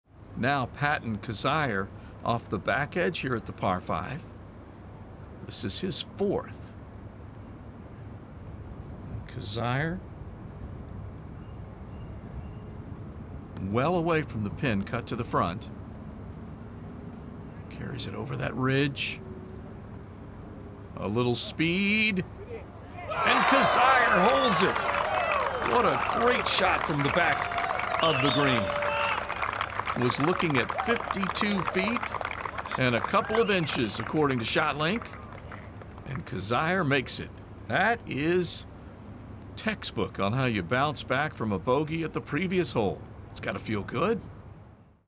Patton Kizzire sinks long-range birdie at RBC Heritage
In the final round of the 2016 RBC Heritage, Patton Kizzire holes a 52-foot birdie putt from the fringe on the par-5 5th hole. .